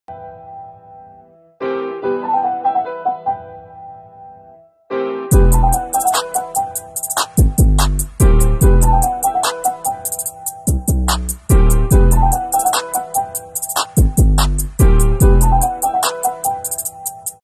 Callisthenics Group Challenge
How strong is your group? This is a challenge for testing the sync and strength in your calisthenics group. Try the workout suggested in the video or come up with a creative one.

Credits - IG - littletfitness